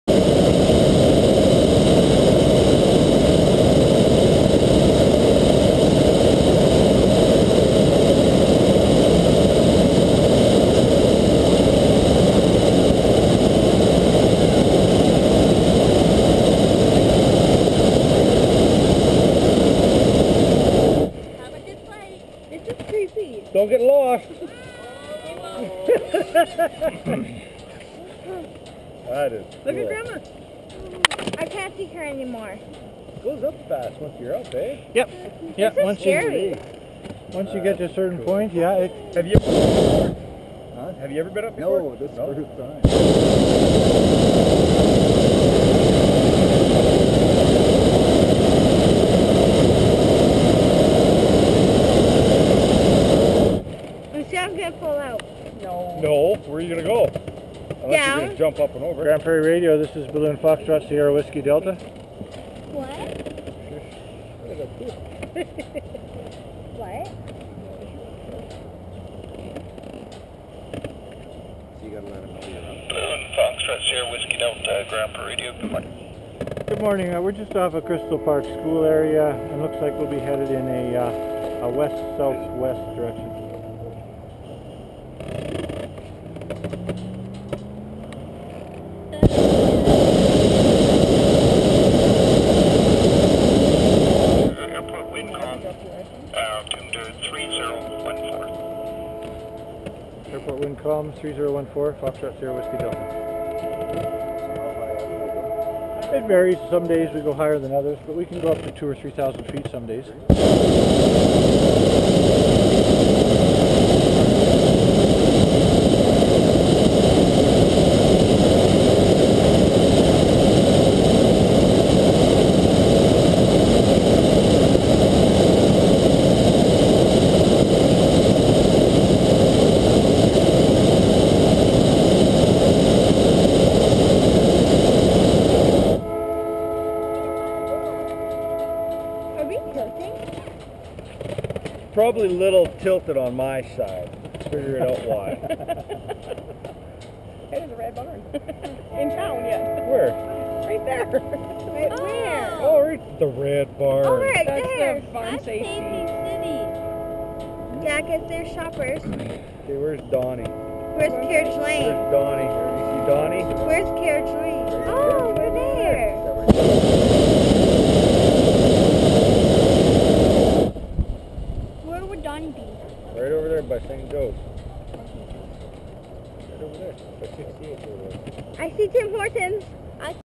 Aug11,2012
Liftoff in a Hot Air Balloon in Grande Prairie on Aug 11 2012.  On an Awesome morning.